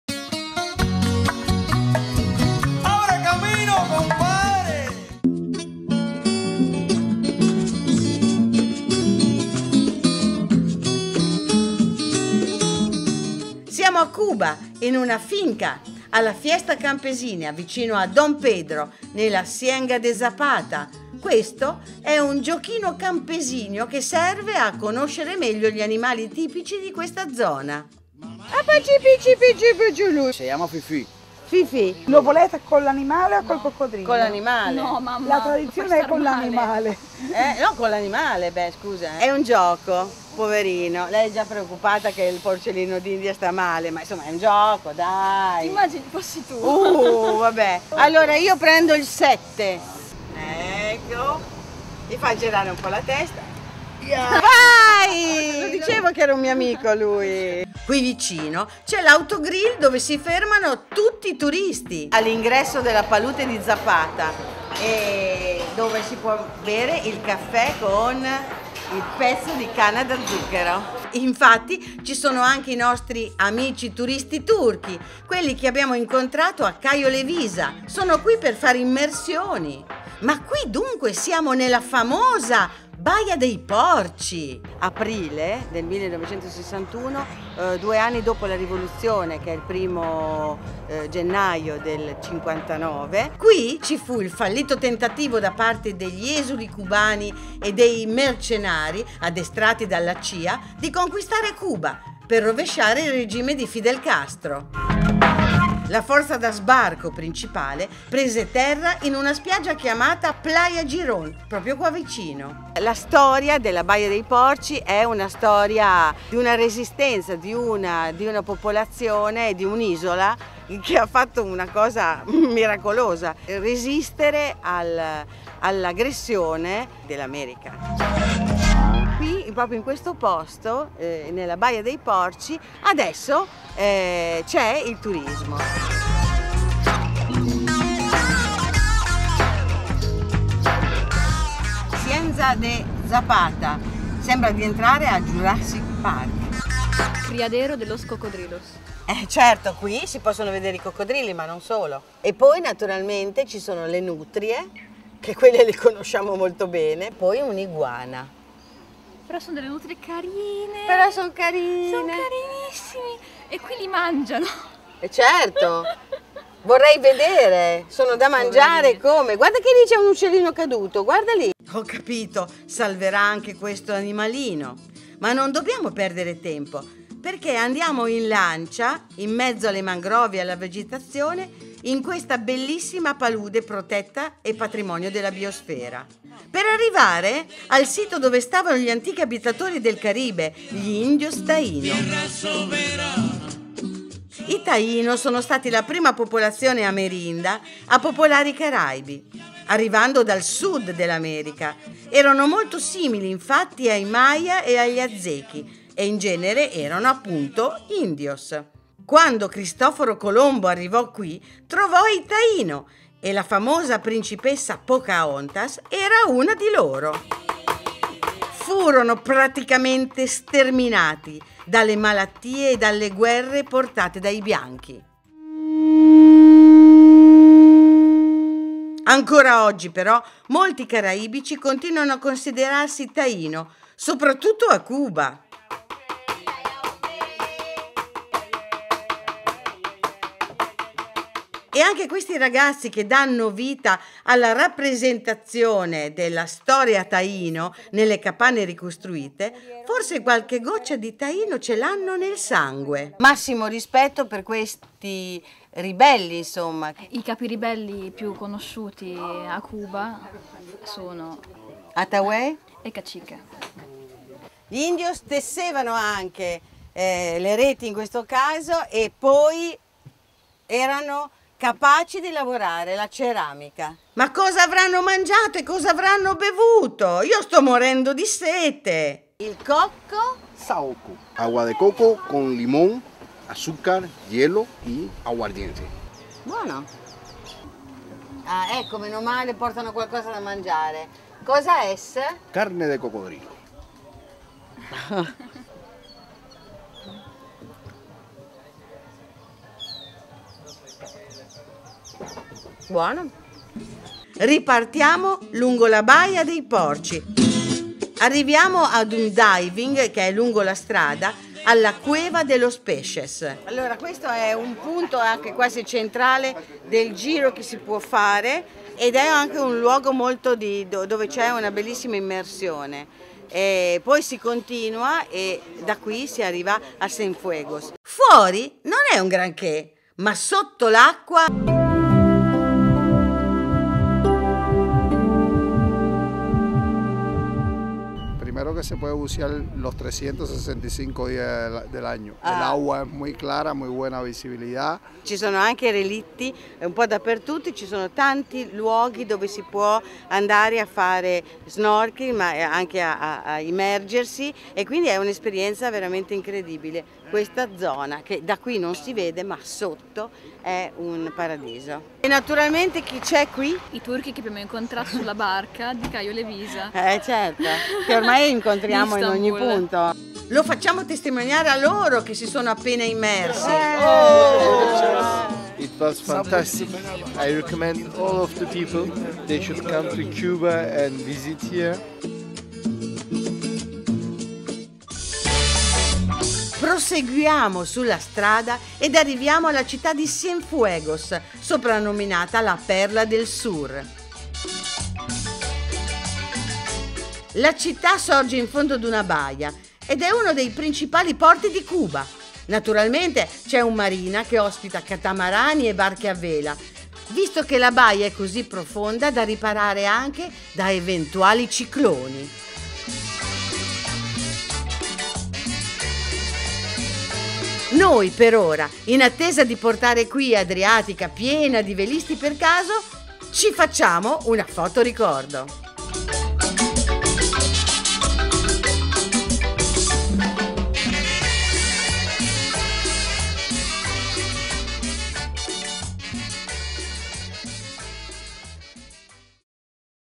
Syusy e Zoe a Cuba: La Baia dei Porci e Cienfuegos | Episodio 6
Martedì 26dicembre, a Santo Stefano, alle 19.30 su Canale Italia seguite lo speciale del mio viaggio in camper a Cuba! Nel frattempo ecco un altro video del viaggio che ho fatto con Zoe qualche tempo fa, siamo al Parco nazionale della Ciénaga de Zapata e alla Bahía de Cochinos o Baia dei Porci, interessanti per più di una ragione: intanto la palude con le sue mangrovie è patrimonio della biosfera e i fondali della baia sono un paradiso per i sub e per lo snorkeling... Storicamente si è consumata qui la più strenua resistenza ai tentativi americani di rovesciare il regime di Fidel Castro. Antropologicamente è la terra degli antichi Indios Taino, popolazione autoctona caraibica sterminata ai tempi dei conquistatori (quelli di Pocahontas, per intenderci...) di cui ancora oggi, con orgoglio, molti abitanti si professano discendenti conservandone tradizioni e costumi. Il viaggio poi prosegue a Cienfuegos, la "perla del Sur".

È proibita la riproduzione, anche parziale, in ogni forma o mezzo, senza espresso permesso scritto dell'autore